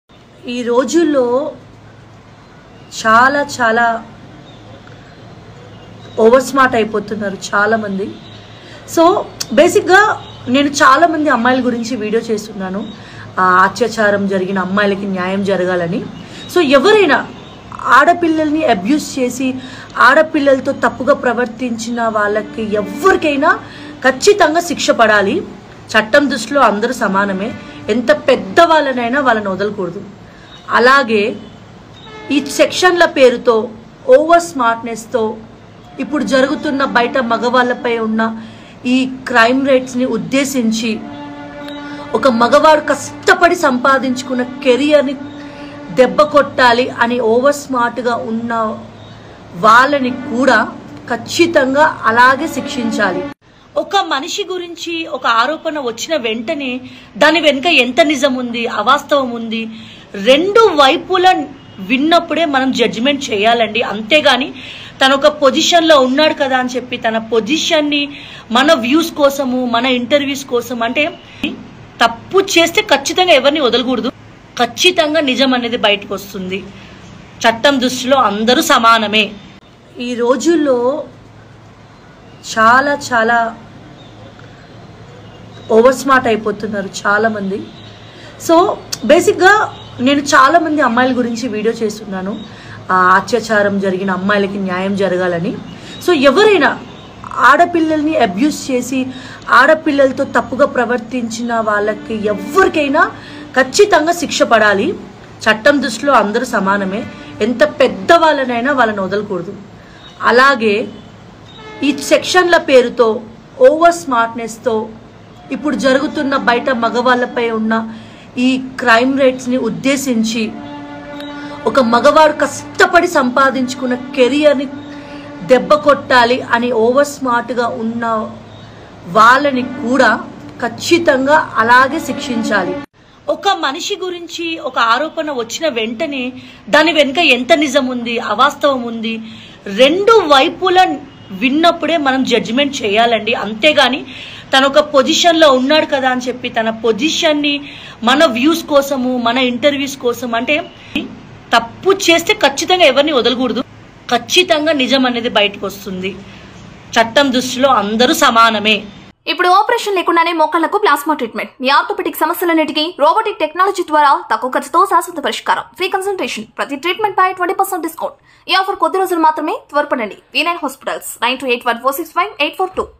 Choreographer Aata Sandeep Wife Jyothi Raj Reaction On Jani Master Issue | News Buzz
Choreographer Aata Sandeep Wife Jyothi Raj Reaction On Jani Master Issue